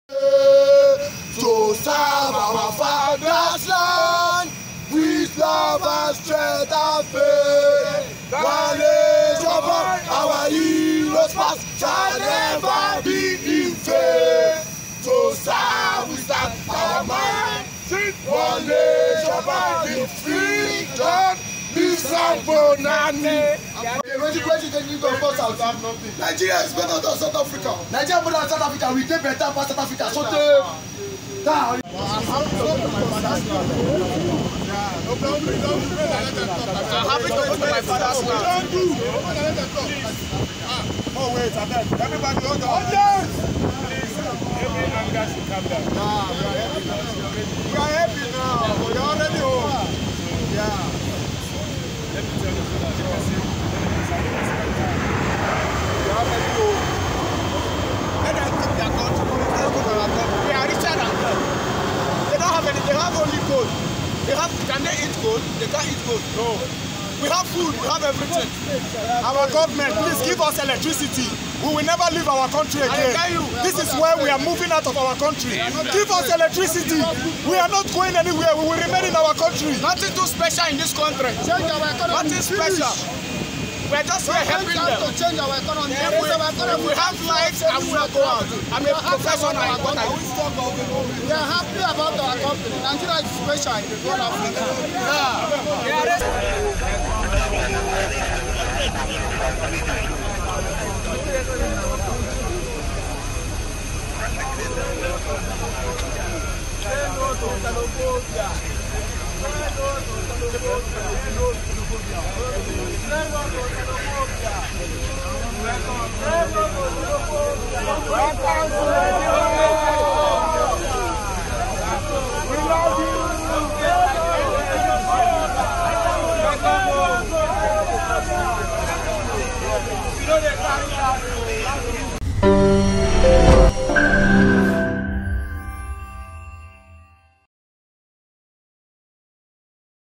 187 Nigerians (1st Batch) Return From South Africa
Returnees state reasons why they fled to South Africa and ask the Government to give steady electricity (power) to make living in Nigeria more worthwhile.